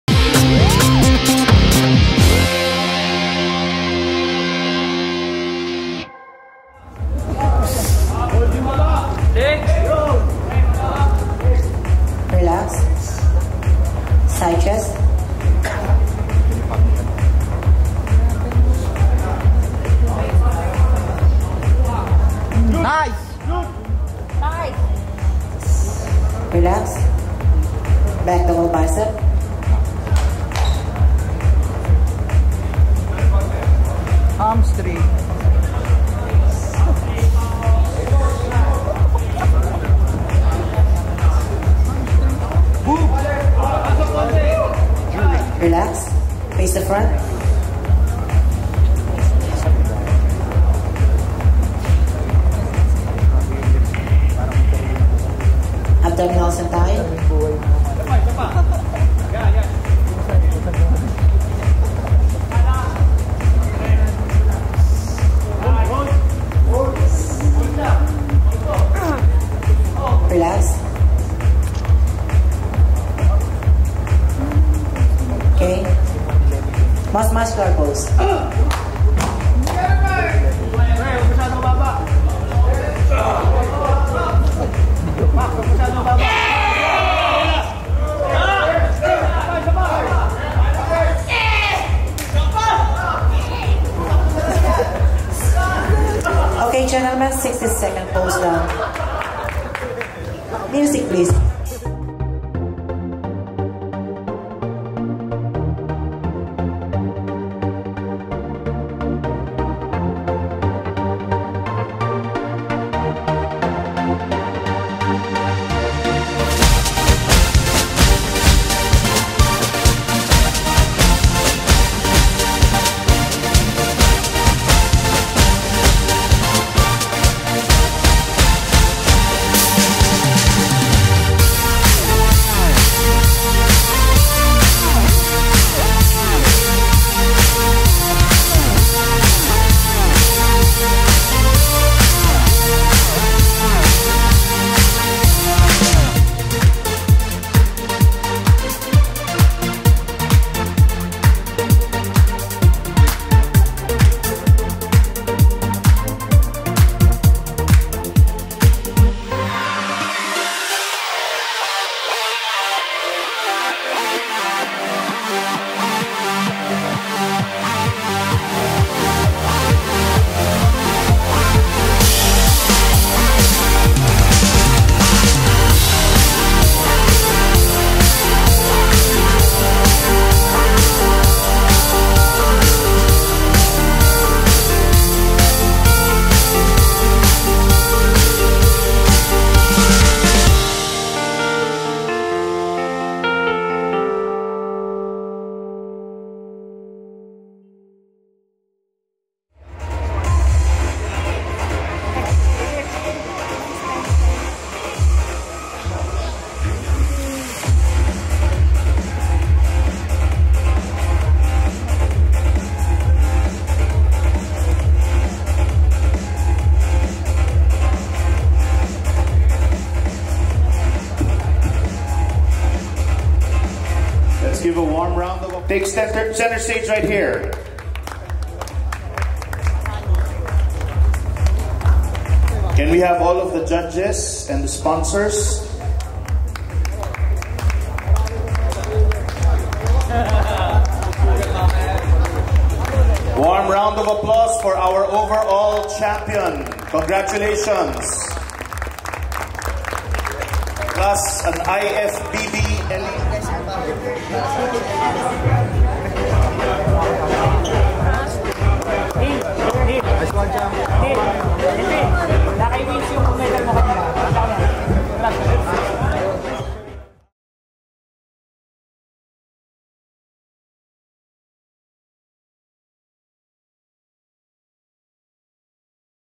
MR PHILIPPINES 2018: Neilvin Dela Pena (Coach Pako Neil) is the Overall Champion
Neilvin Dela Pena - Tag 110 (Coach Pako Neil) is Overall Champion of Mr Philippines 2018
Congratulations!